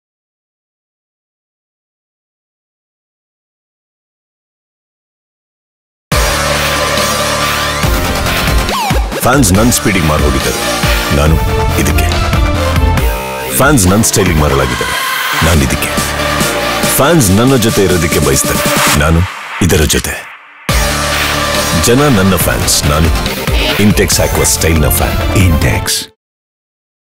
Intex Aqua Style 20sec Kannada
Featuring Kannada superstar Kiccha Sudeep